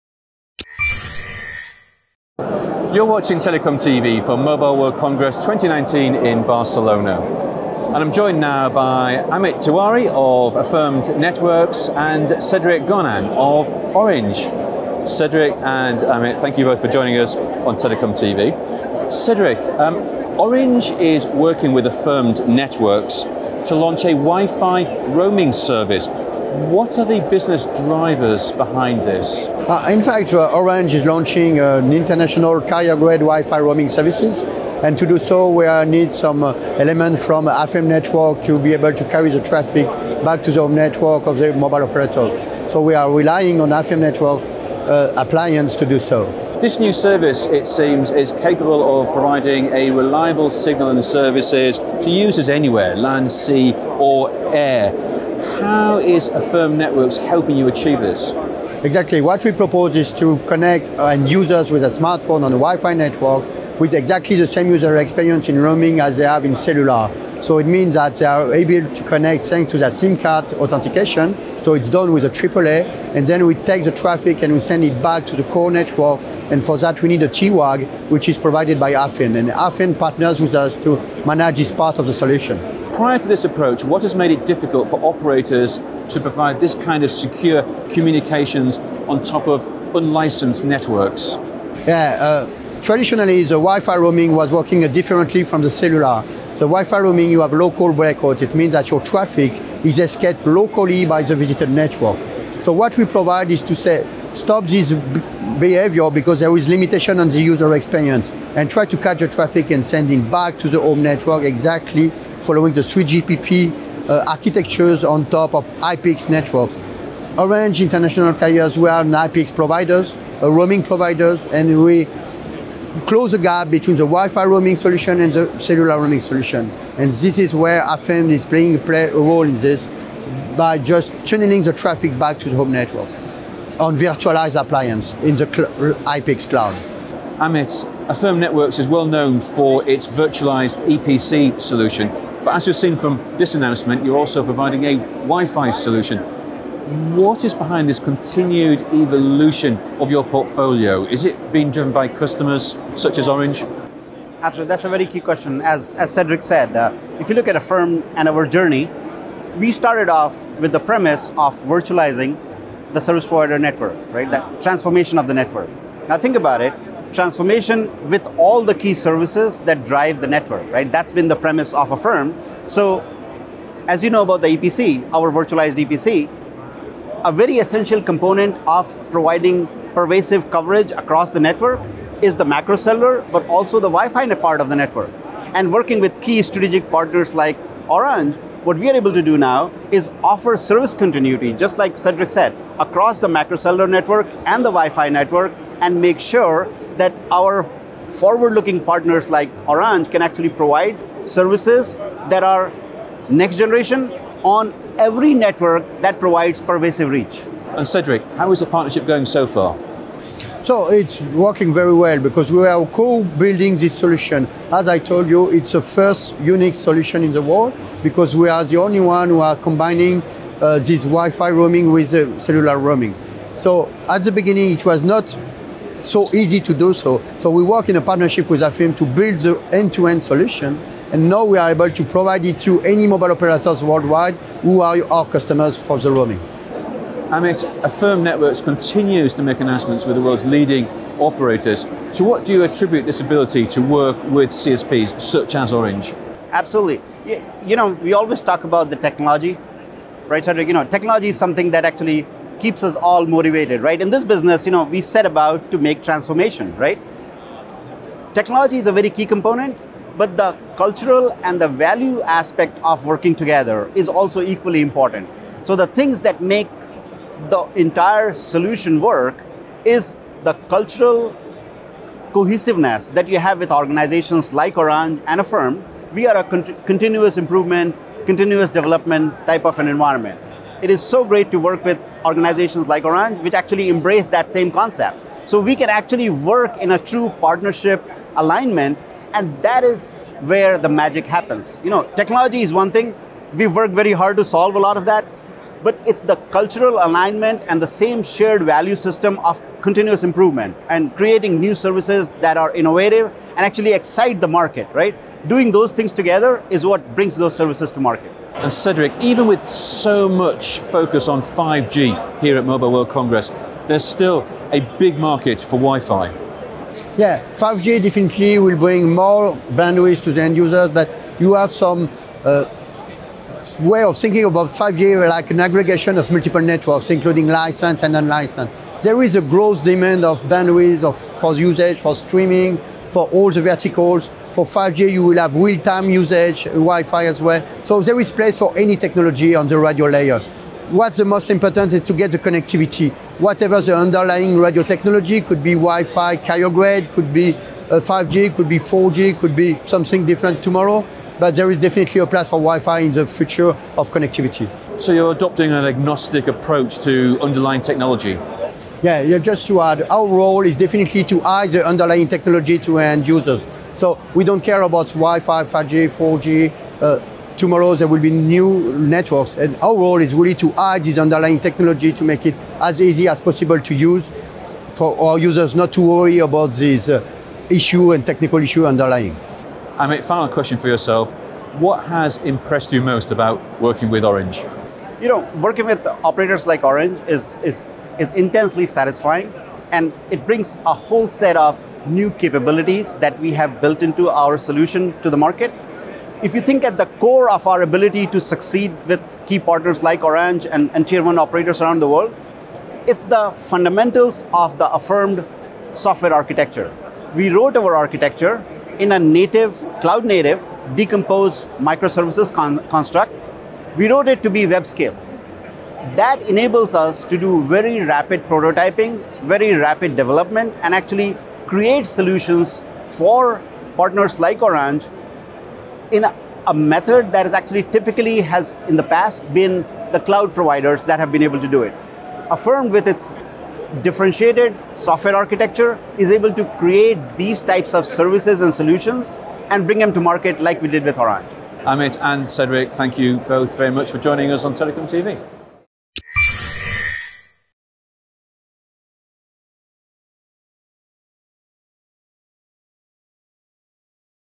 Affirmed and Orange partner on Wi-Fi roaming
Orange is working with Affirmed Networks to launch a Wi-Fi roaming service, providing secure and reliable connectivity over unlicensed spectrum networks. Affirmed is building on its success in virtualised EPCs to expand its innovative solutions into new markets to support its CSP customers and their business needs. The new solution proves that even with 5G services coming to market, there is still a big future for Wi-Fi and other wireless technologies.
Featuring: Cedric Golin, Senior Director of Marketing, Mobile Services, Orange International Carriers
Amit Tiwari, VP Strategic Alliances and Systems Engineering, Affirmed Networks
Filmed at: MWC19, Barcelona